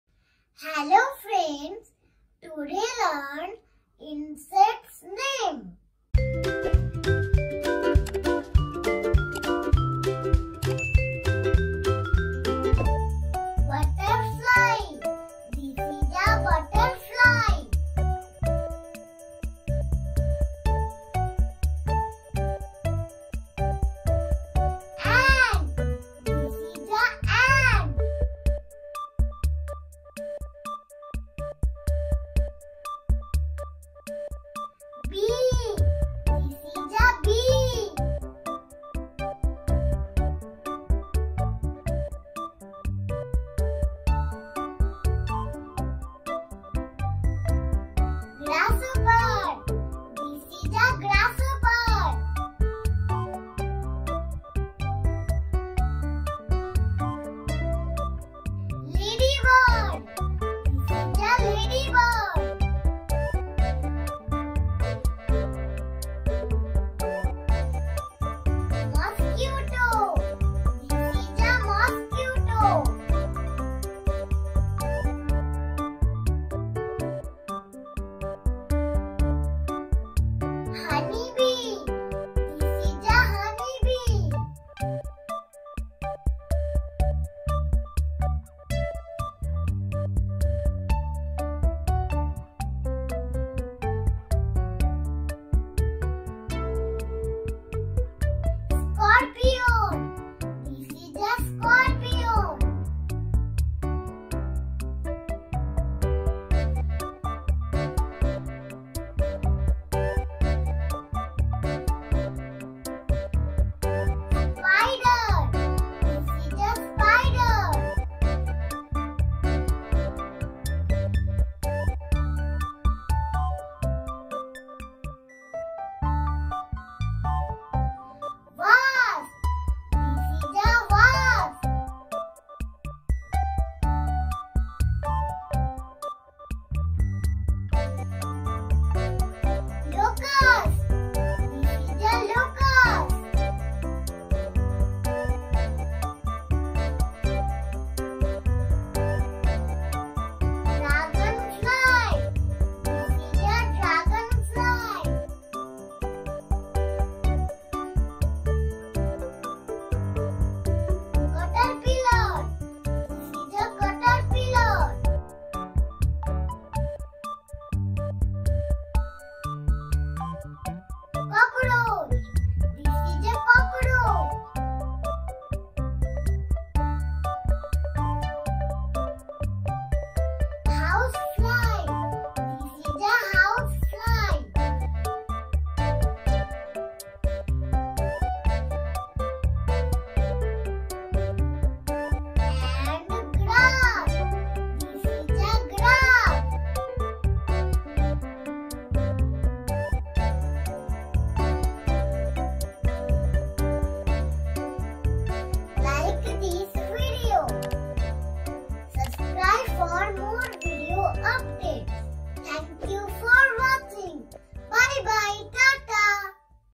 Common Insects Names for Kids to Learn
"Fun and Educational: Common Insects Names for Kids to Learn" - Join us as we explore the world of insects and learn about their names, habitats, and unique characteristics. This video is perfect for young learners who love to explore the great outdoors!